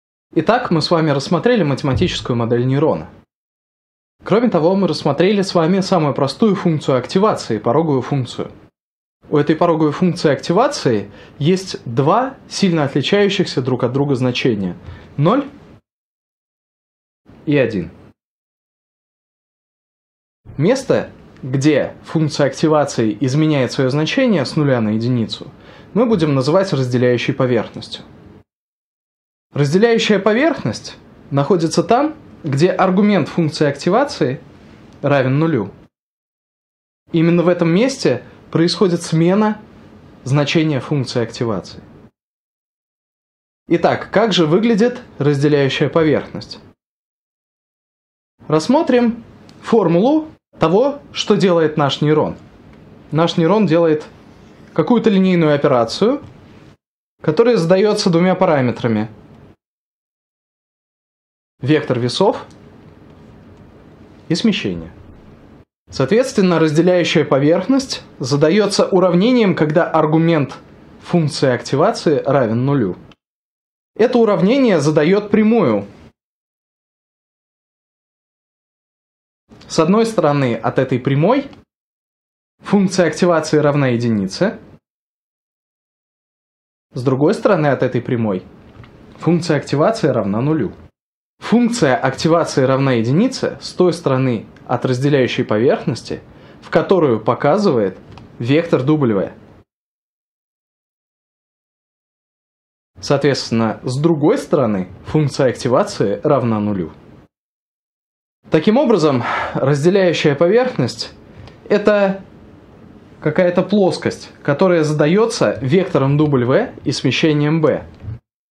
Нейронные сети и компьютерное зрение - 5 урок. Математическая модель нейрона
Автор: Михаил Романов, Игорь Слинько, Николай Капырин

Задание на закрепление:

Какие значения выдает пороговая функция активации?

Выберите все подходящие ответы из списка:

* 0, 1

* Все вещественные числа

* 0, 1, 2, 3

* -1, 1

* e^xe
* x